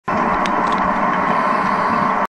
SDV 0030 Suspect alien craft,lw down to left.
This video I'll re-do in slow motion and shows a small saucer 2mls NE OF me .Wed.11sept.2013,have added a custom thumb,and will remake digitally shortly,to prove alien craft!Robert Newton,19mls.W,of Manchester.